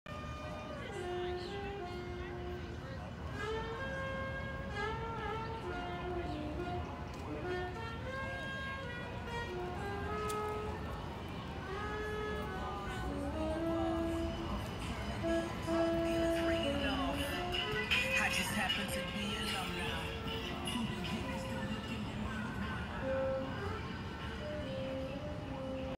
Chippewa Square, Savannah Georgia
Jazz music in the background and across from the Savannah Christian Church